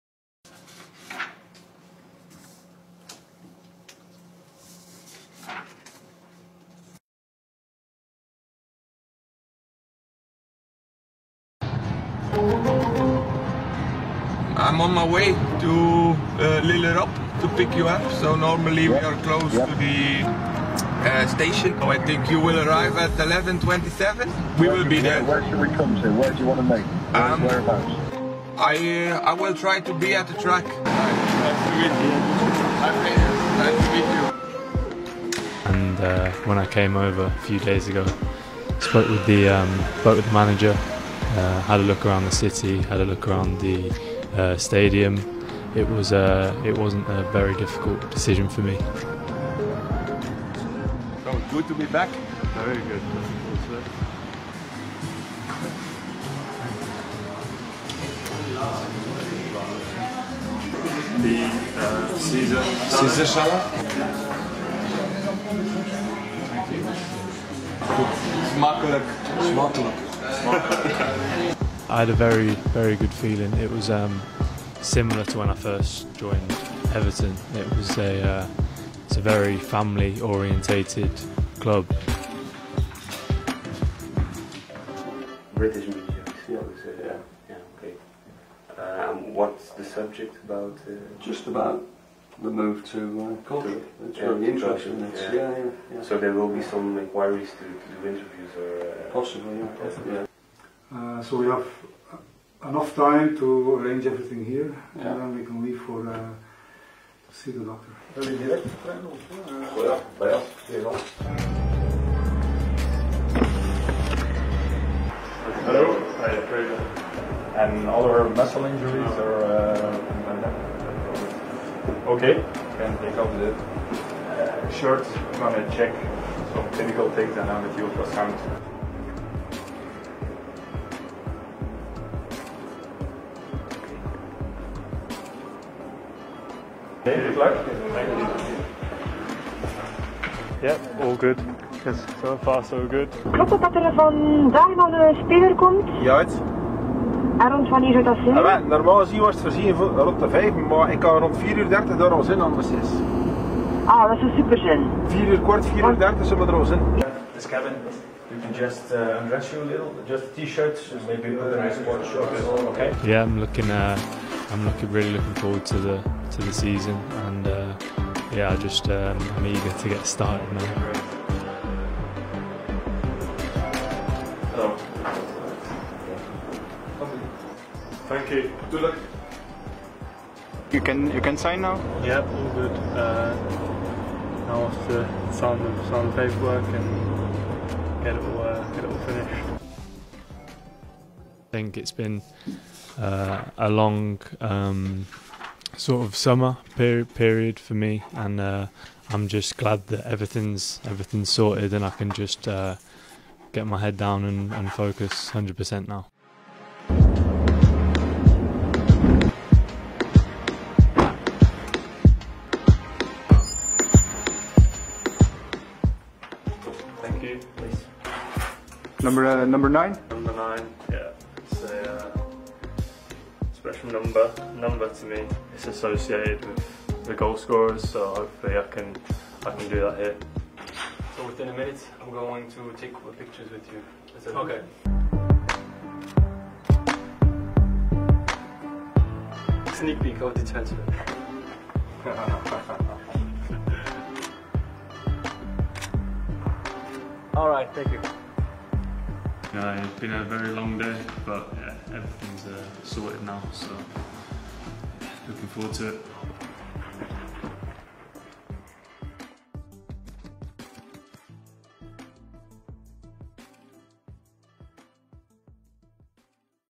Eerste dag van Fraser Hornby op KV Kortrijk
De dag van de transfer van Fraser Hornby naar KV Kortrijk. 🔴⚪

Everton Football Club ➡ KV Kortrijk